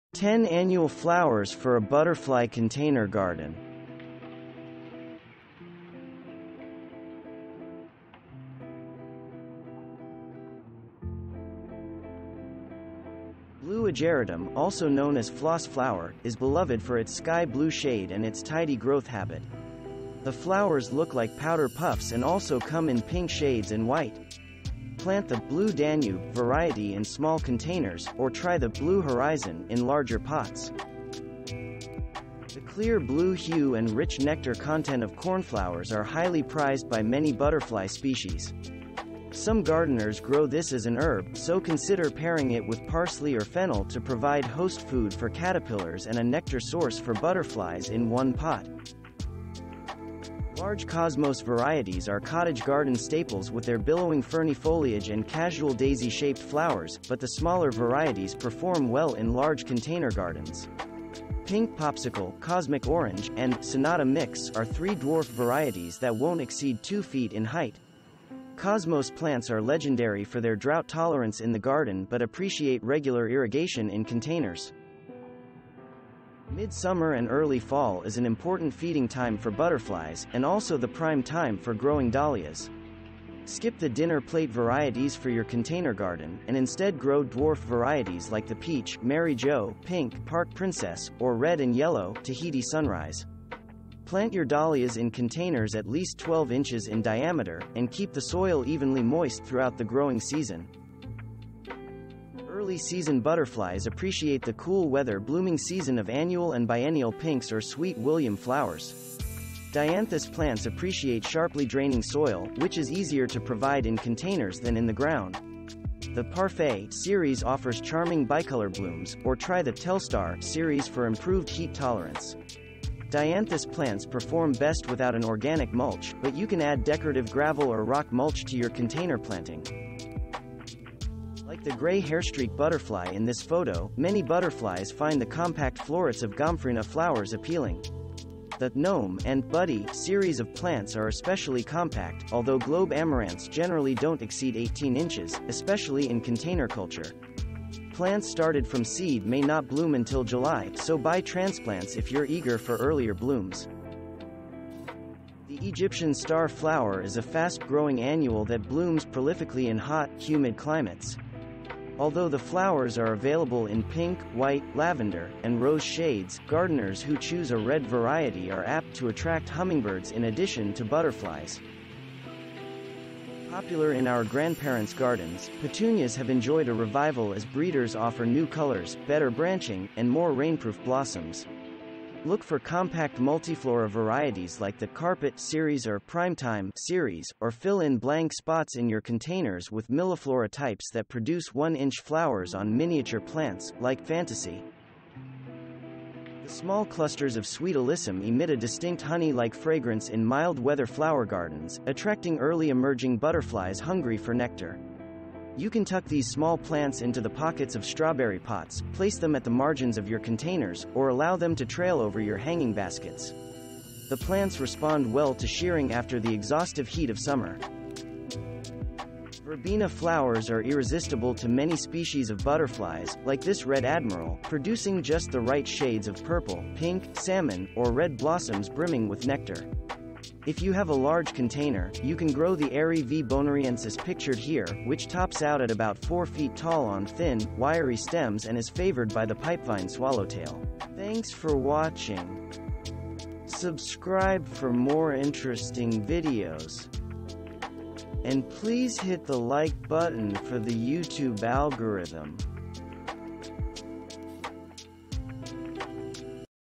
10 Annual Flowers For A Butterfly Container Garden 🛋️
Even small landscapes can attract wildlife with flowers. Bring on the butterflies to gardens of any size with nectar-rich container gardens.
0:00 - Intro
0:00:12 -  Ageratum
0:00:21 -  Bachelor's Button
0:00:30 -  Cosmos
0:00:39 -  Dahlia
0:00:48 -  Dianthus
0:00:57 -  Globe Amaranth
0:01:06 -  Penta
0:01:15 -  Petunia
0:01:24 -  Sweet Alyssum
0:01:33 -  Verbena